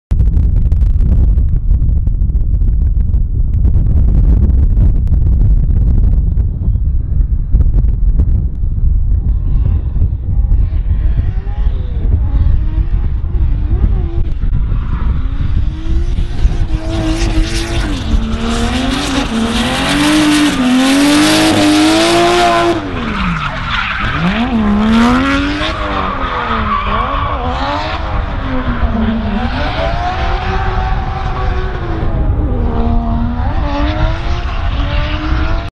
Julian Jacobs - Testing at Sonoma Raceway
Julian testing out his 1jz Formula Drift s14. 4/17/13